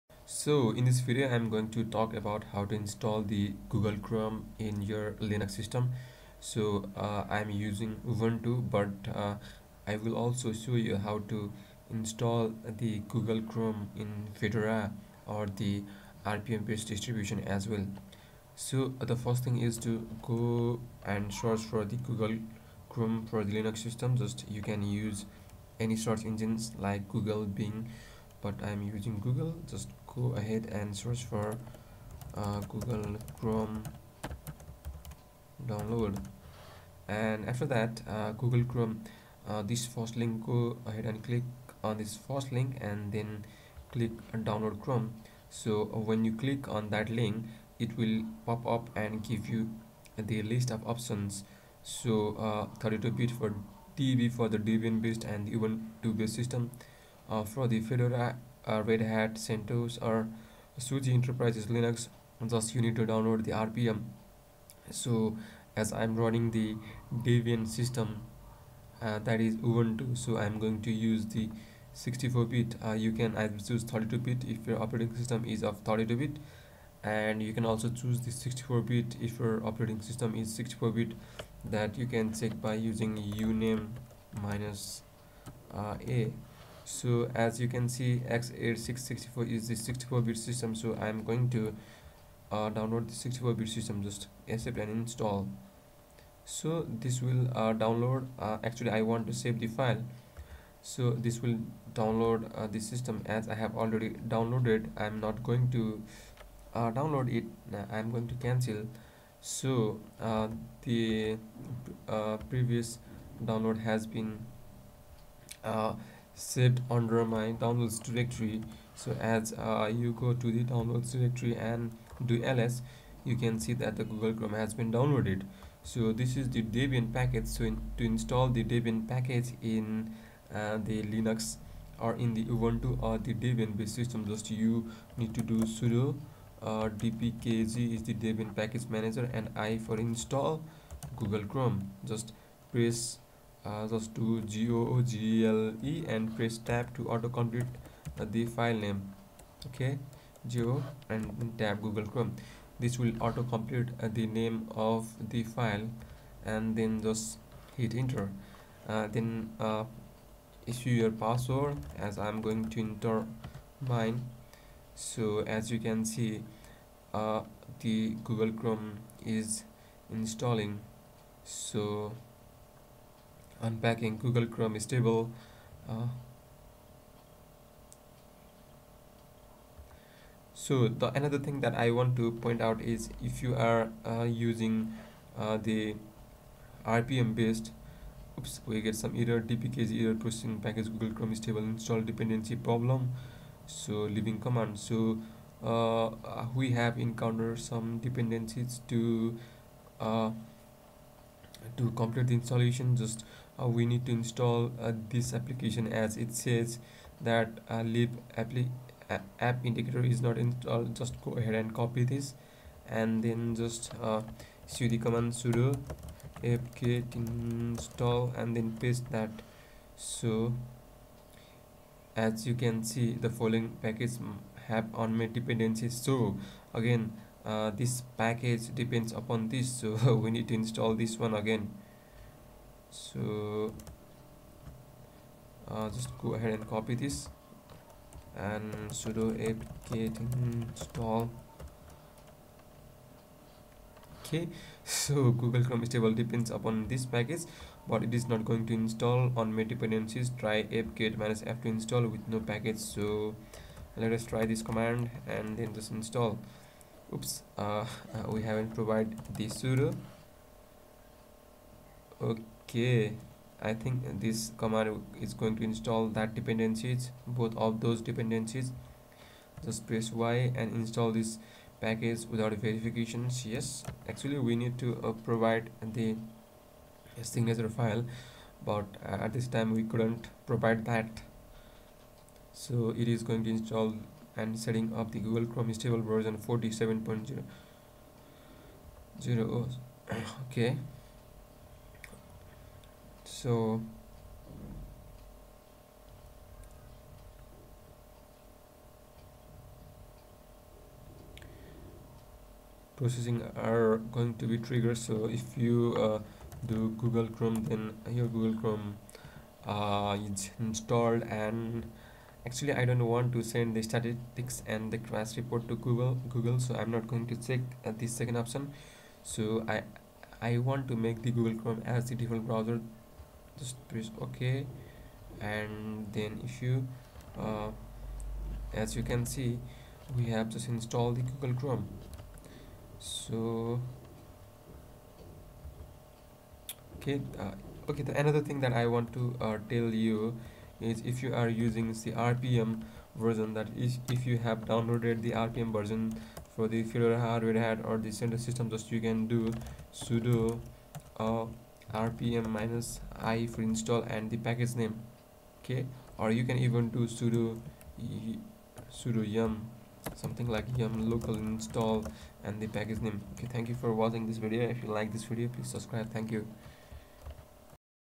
Installing and running google chrome in Linux (Ubuntu 15.04 / Fedora /Suse)
Installing google-chrom in linux (ubuntu 14.04/15.04/16.04, fedora 23 suse)Google Chrome is a freeware web browser developed by Google. It used the WebKit layout engine until version 27 and with the exception of its iOS releases, from version 28 and beyond uses the WebKit fork Blink. It was first released as a beta version for Microsoft Windows on September 2, 2008, and as a stable public release on December 11, 2008.

As of December 2015, StatCounter estimates that Google Chrome has a 58% worldwide usage share of web browsers as a desktop browser. It is also the most popular browser for smartphones, and combined across all platforms at about 45%. Its success has led to Google expanding the 'Chrome' brand name on various other products such as the Chromecast.

Google releases the majority of Chrome's source code as an open-source project Chromium. A notable component that is not open source is their version of the built-in Adobe Flash Player, called Pepper Flash Player.